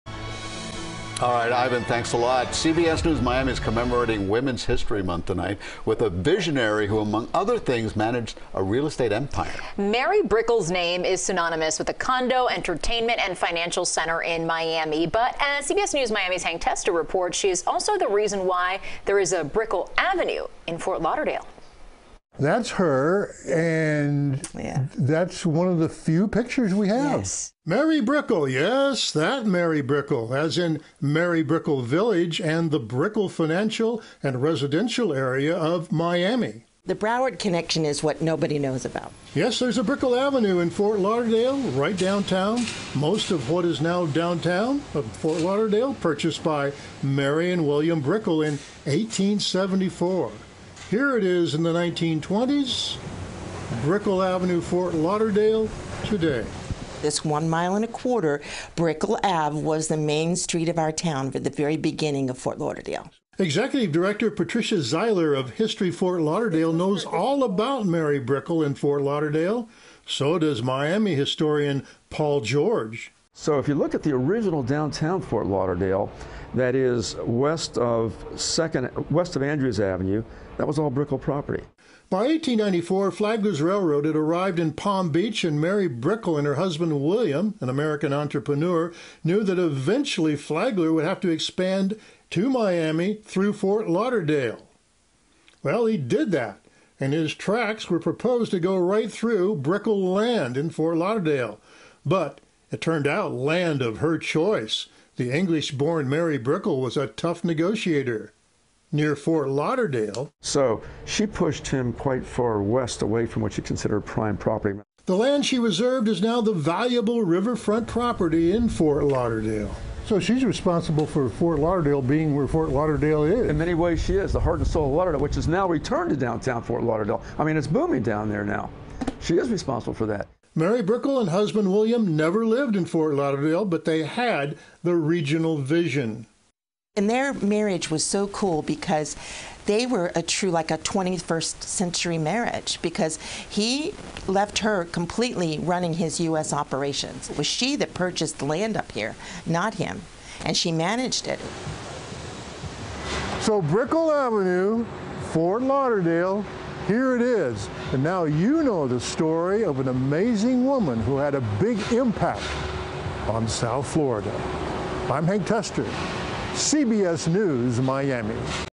Did you know? Mary Brickell was instrumental in shaping Fort Lauderdale
Most of what became downtown Fort Lauderdale was purchased by Mary and William Brickell in1874